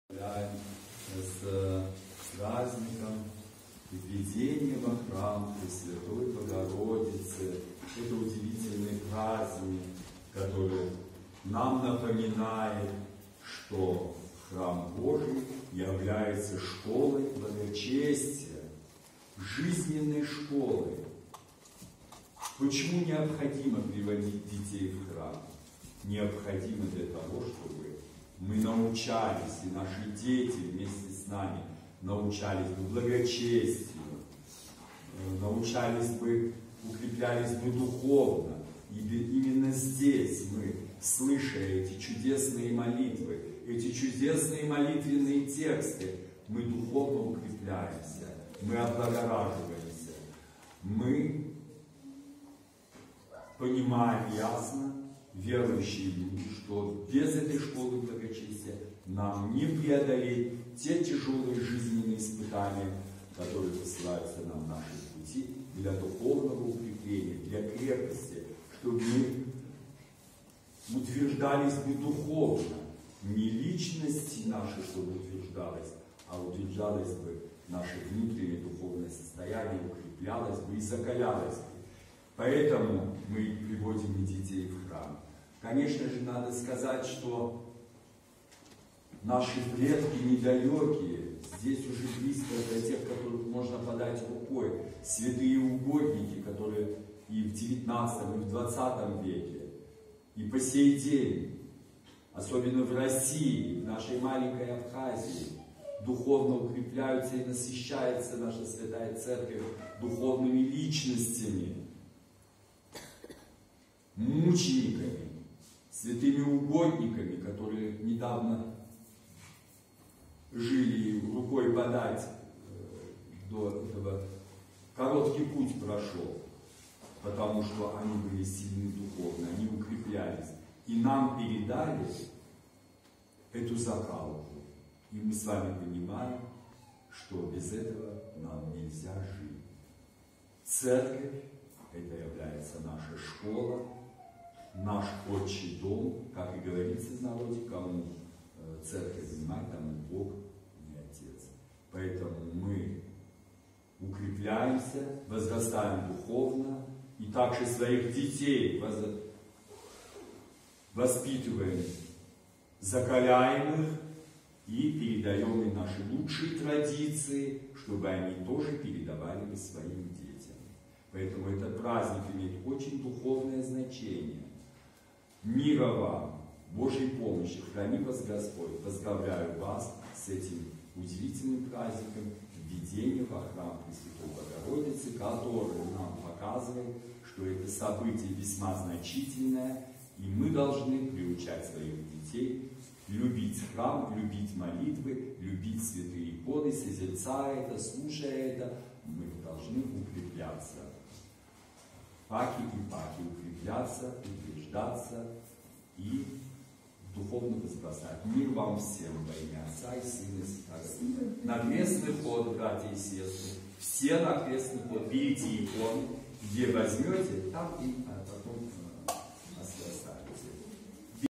КАМАНЫ 3.12.2022 Слово настоятеля к празднику ВВЕДЕНИЯ ВО ХРАМ Пресвятой Богородицы
Слово настоятеля монастыря Иоанна Златоуста игумена Игнатия (Киут) к празднику ВВЕДЕНИЯ ВО ХРАМ Пресвятой Богородицы . Каманы, Абхазия